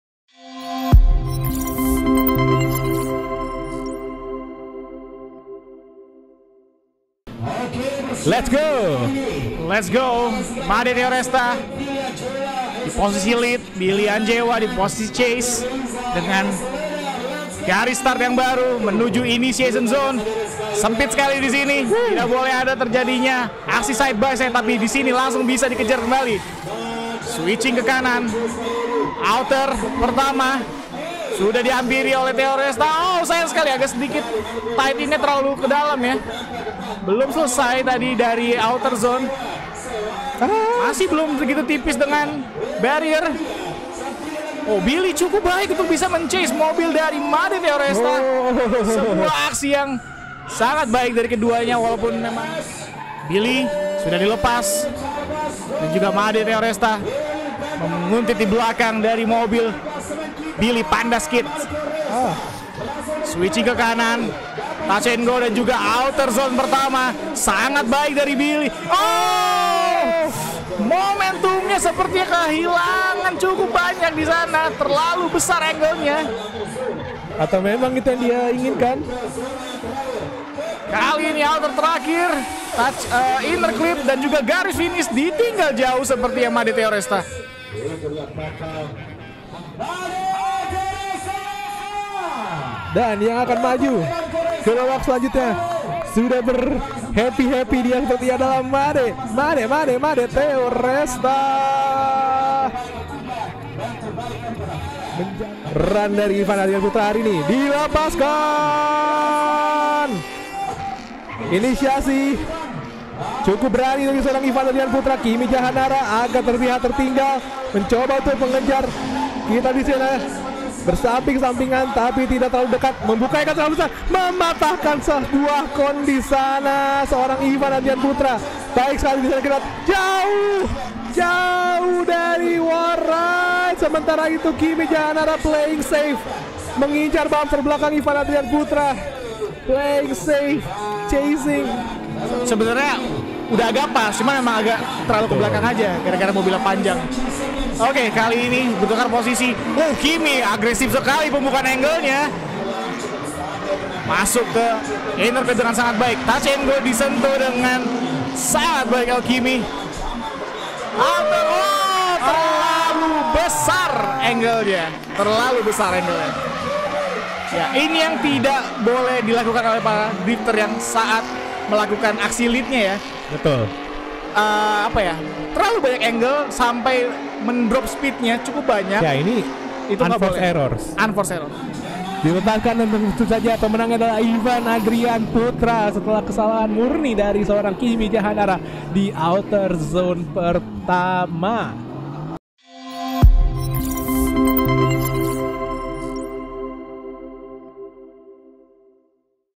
HIGHLIGHTS - SEMIFINAL PRO2 || Indonesian Drift Series Final Round 2022
Tandem Battle Made Theoresta vs Billy Anjewa & Kimi Jahanarra vs Ivan Adrian

Be a professional with us.

Powered by BMW Original Parts.

Nge-drift aman? Pakai Accelera!
Ingat velg? Ingat HSR!